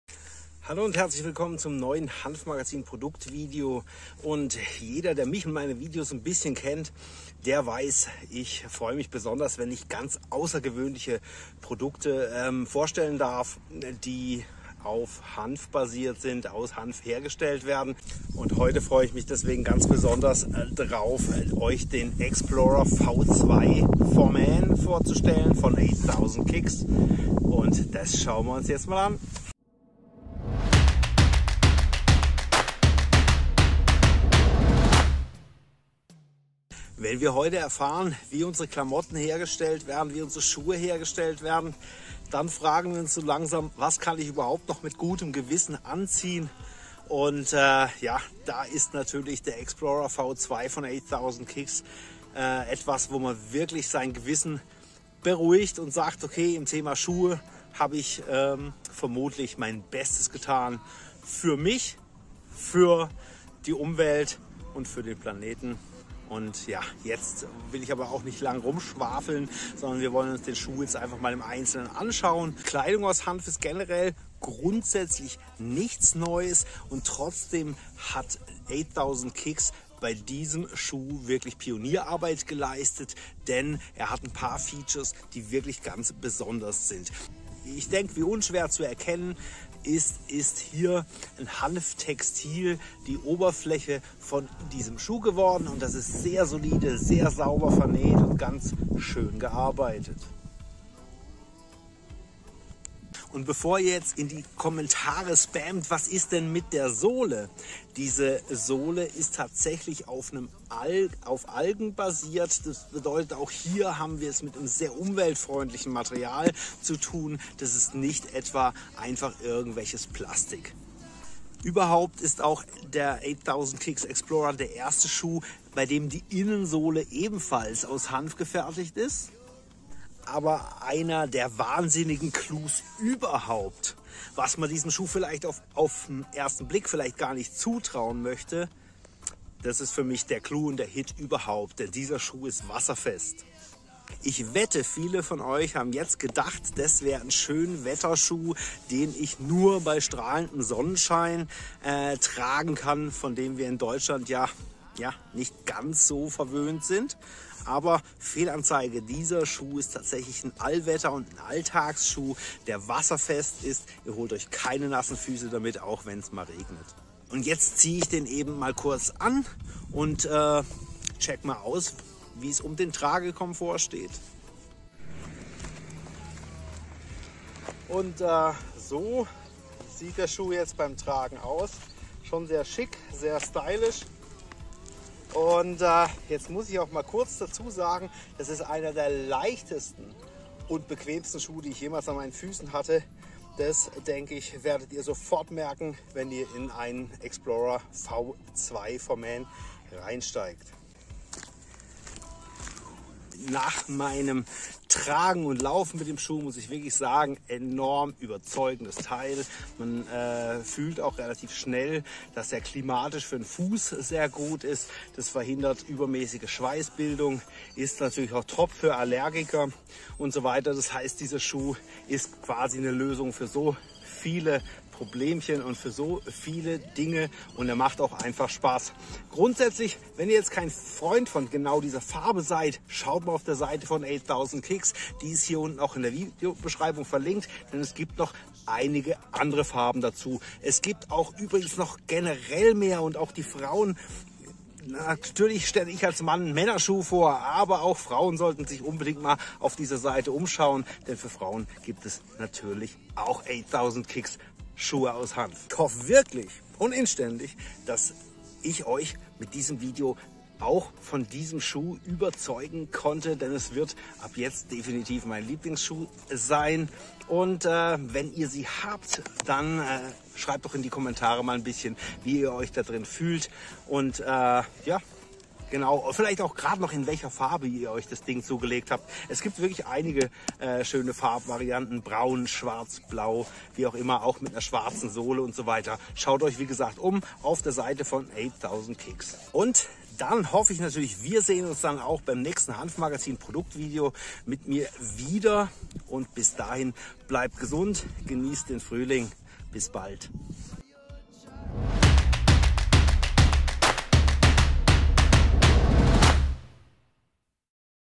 Hanf-Sneakers von 8000 Kicks
Tipp für die Bestellung: Der Schuh fällt ziemlich klein aus, am besten eine Größe über eurer gewohnten nehmen, also z.B. 43, wenn Ihr sonst 42 habt.